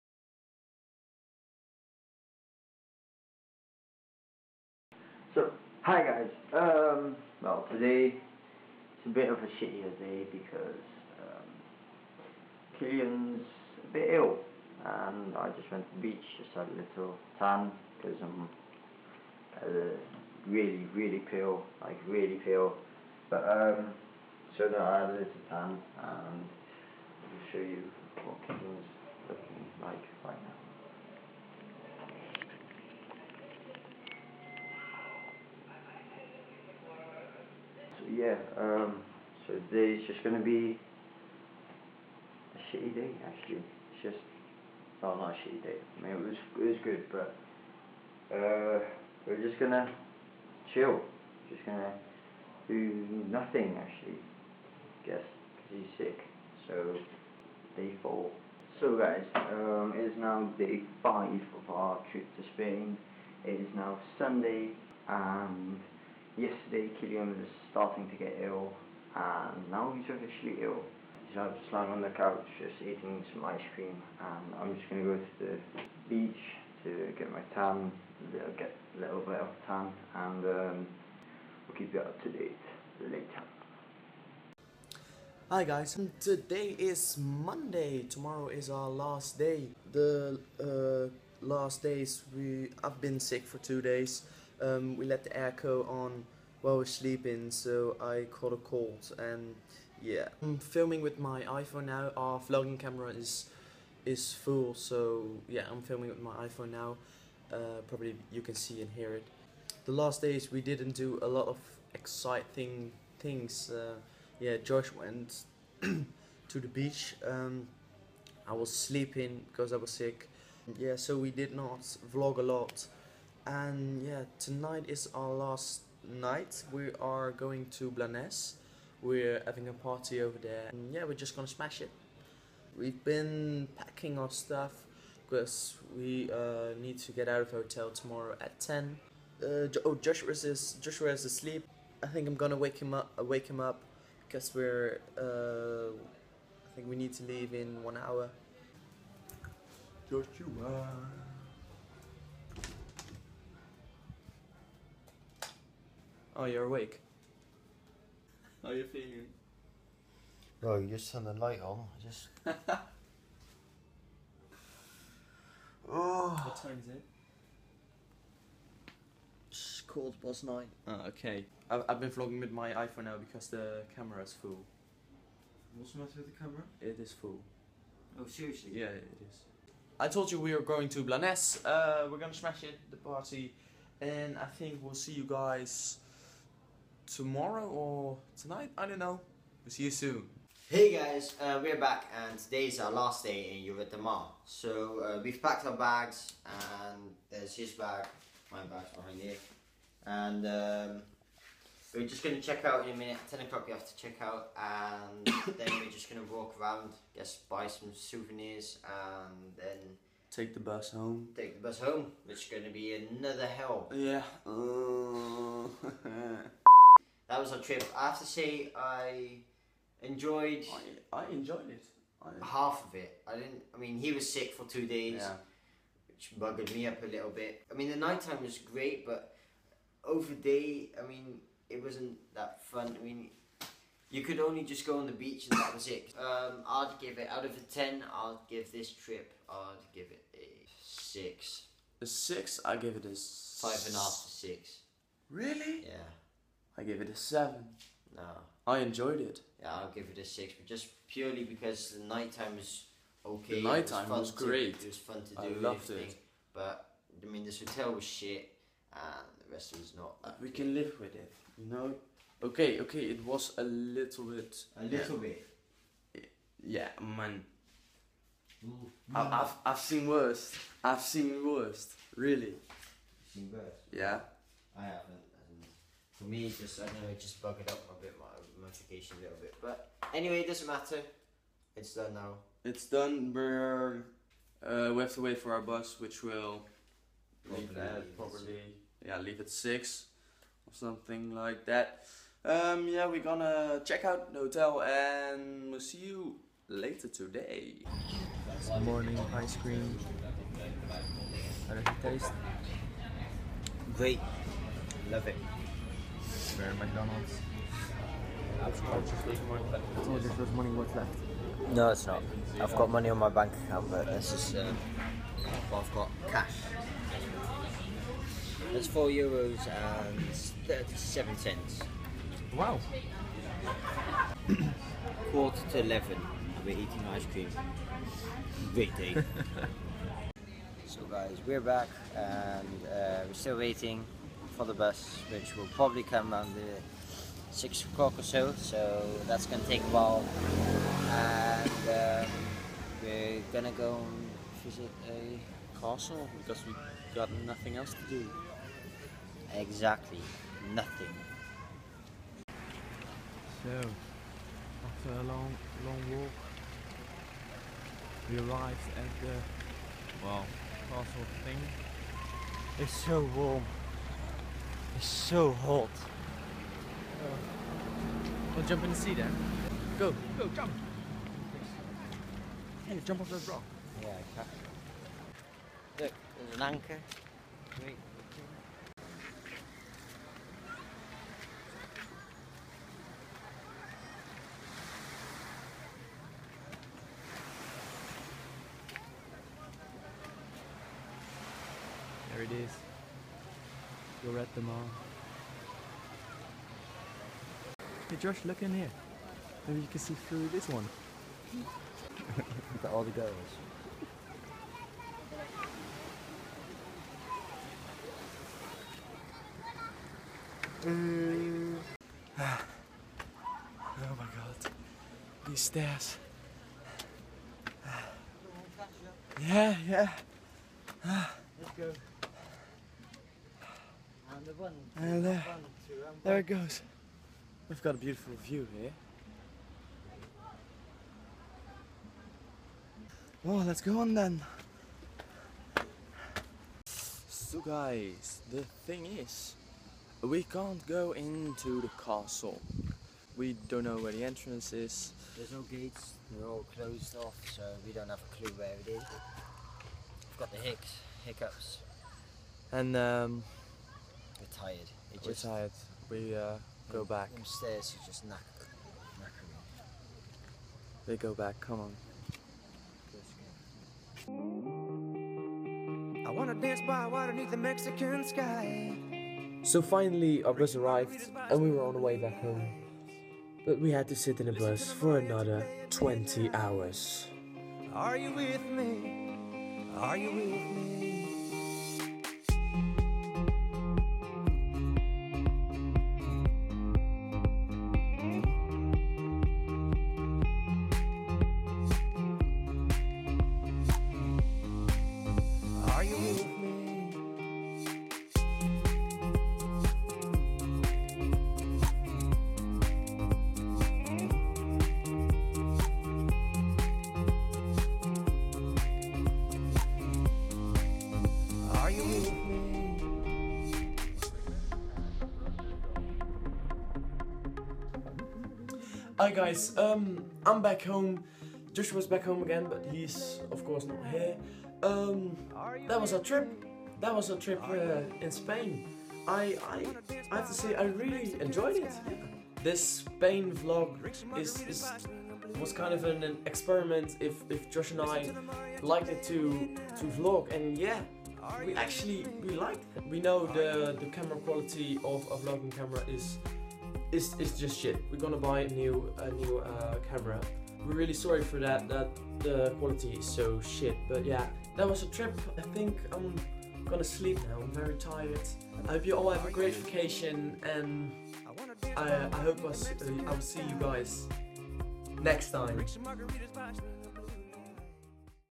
Happily coming home - Spain vlog #3
Hey guys,

Todays video is about our last days in Spain!

joshuaweelen
kilian1212